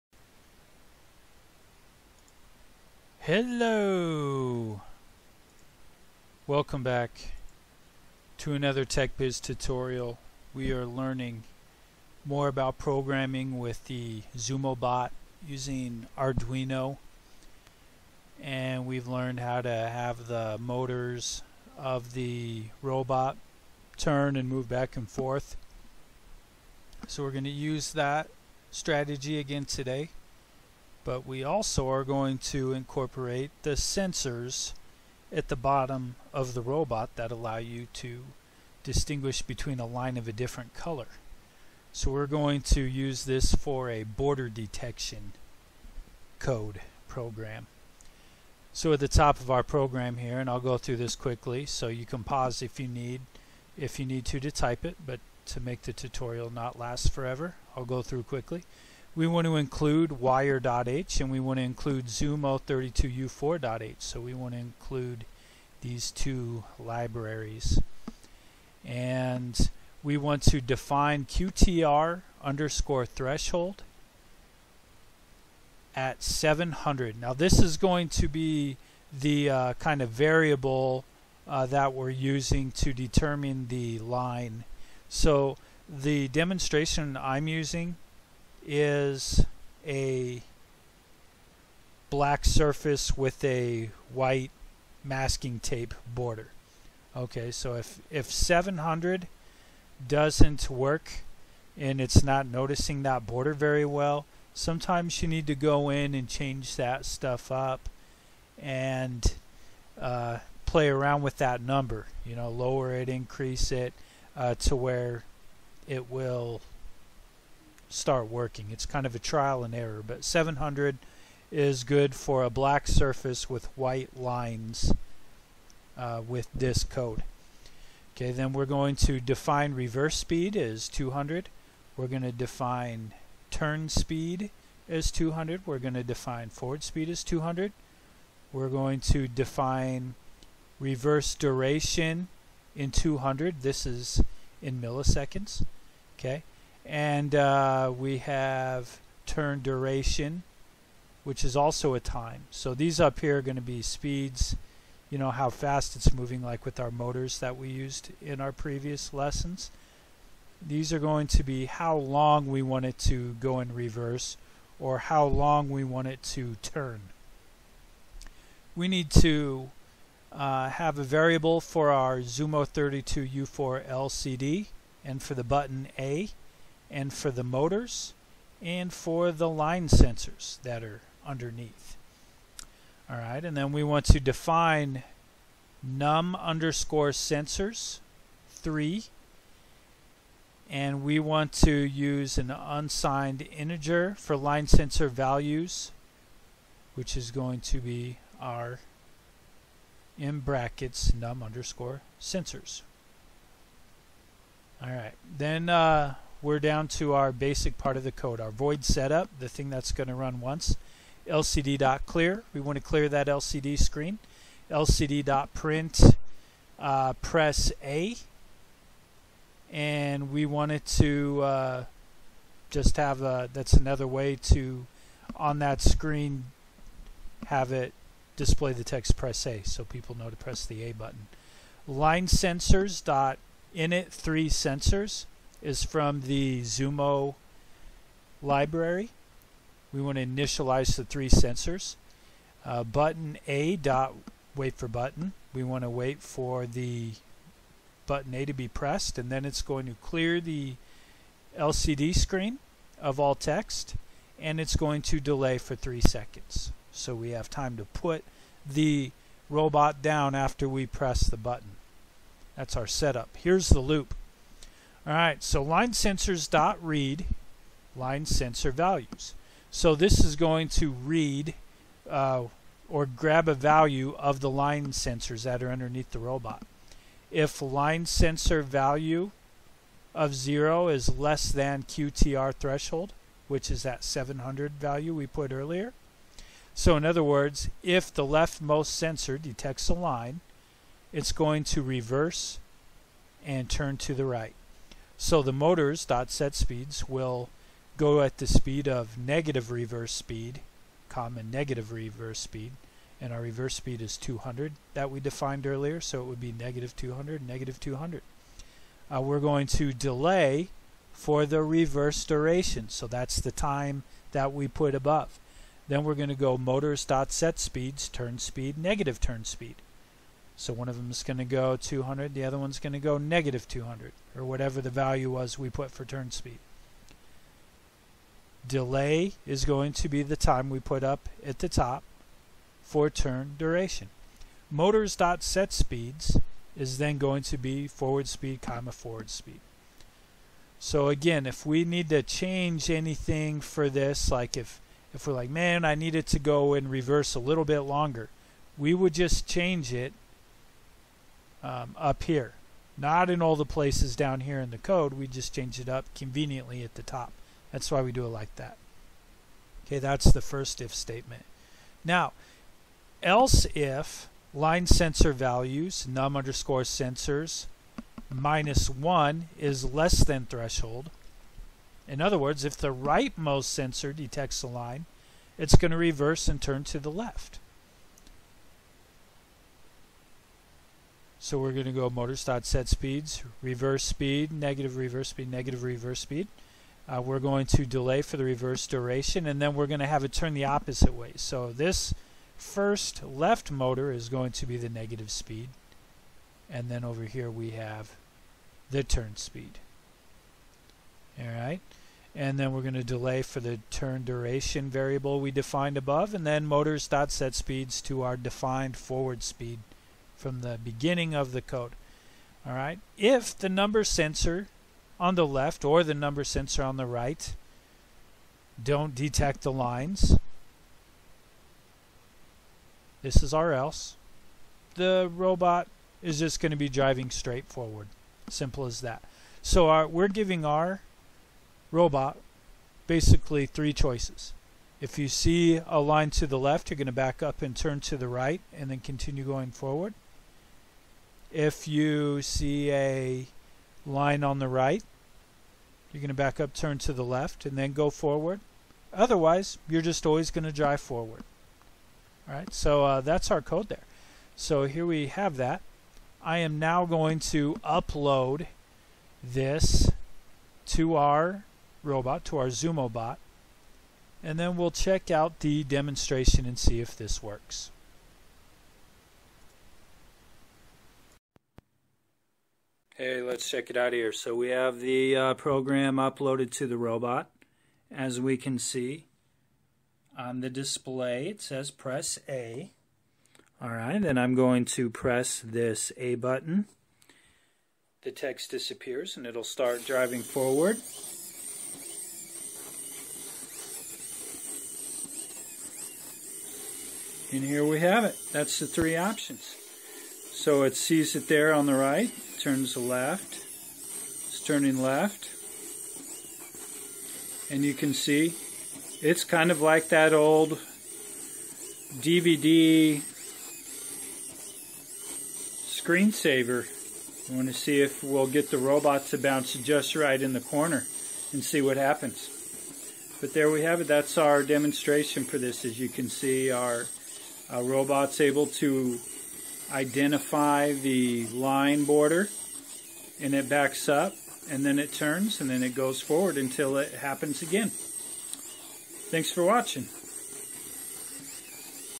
Zumo Bot Arduino 8: Line Detection
Using Arduino language, we are programming our line sensors on our Zumo bots!